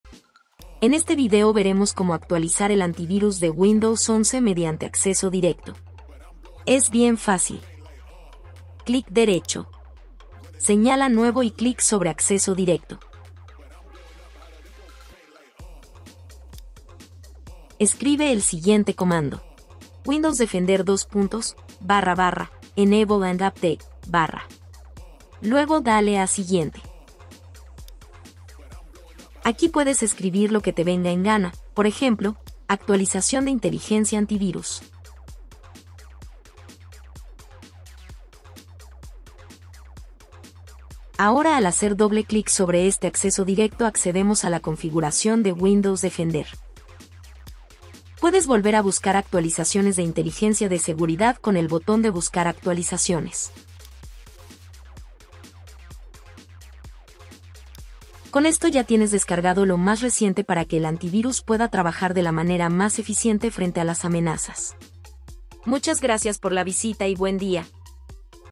✅ Acceso directo ➡️ actualizar Windows Defender manualmente 🎯
💥Actualizar el antivirus de Windows 11 es un proceso sencillo y rápido que ayuda a mantener tu sistema seguro. El comando proporcionado, `windowsdefender://enableandupdate/`, es un método directo para habilitar y actualizar Windows Defender, el antivirus integrado en Windows 11. Al crear un acceso directo con este comando, puedes actualizar fácilmente la inteligencia de seguridad con solo un par de clics. Este proceso garantiza que tu antivirus esté equipado con las últimas definiciones de virus, lo que es crucial para proteger tu computadora contra las últimas amenazas detectadas. Recuerda que mantener tu software de seguridad actualizado es una de las prácticas más importantes para la seguridad informática.